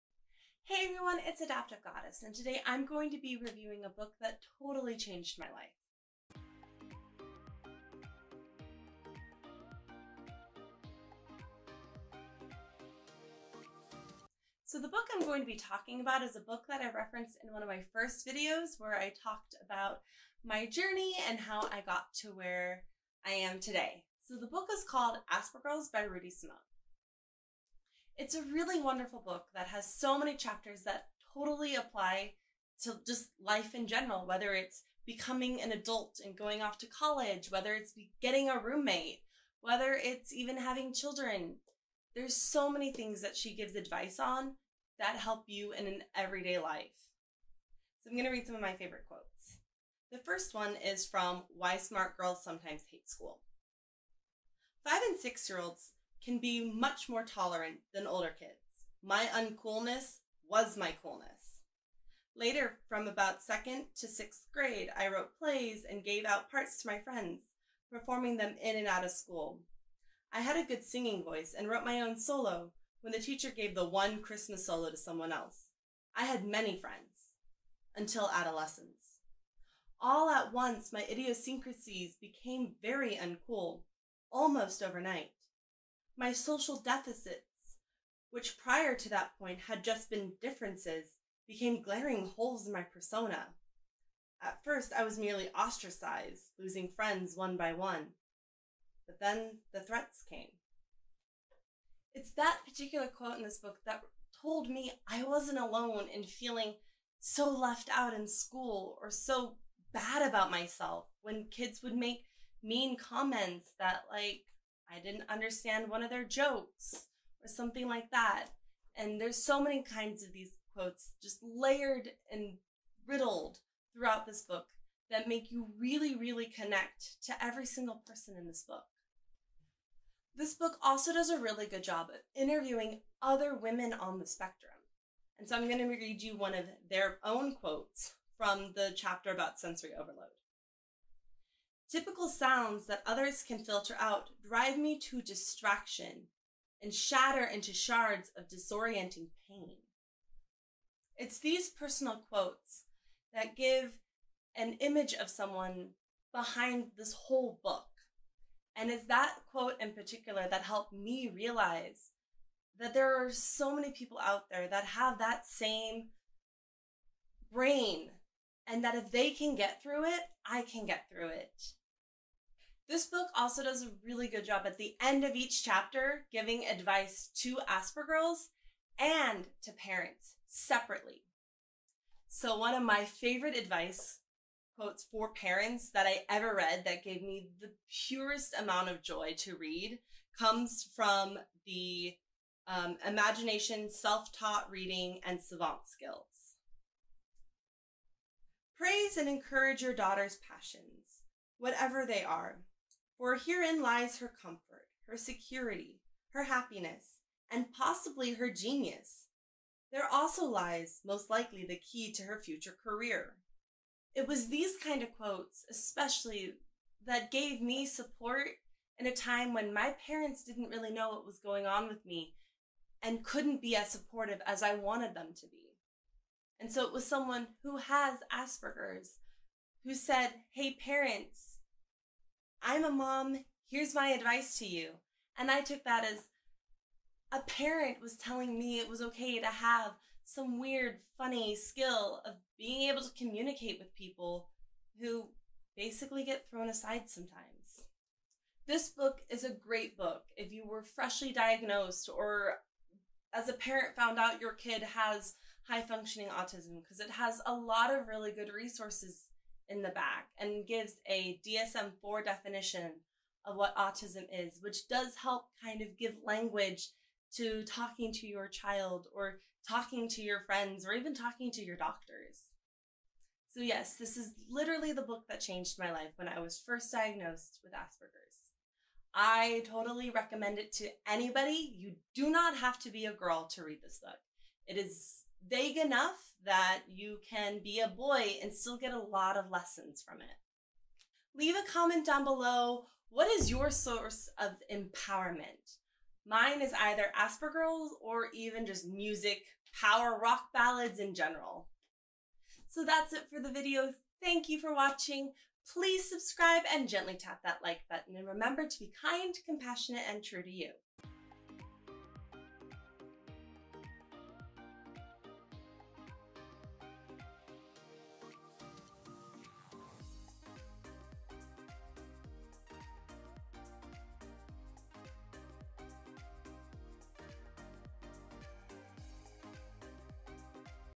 An Aspergirl Views on Aspergirls by Rudy Simone
I review the book Aspergirls by Rudy Simone. This book really helped me understand myself and why I wasn't really fitting in. I give a personal perspective of how this book can change your life. I recommend this book to anyone parents included as it give resources and is a great place to start understanding Autism Spectrum Disorder and sensory processing.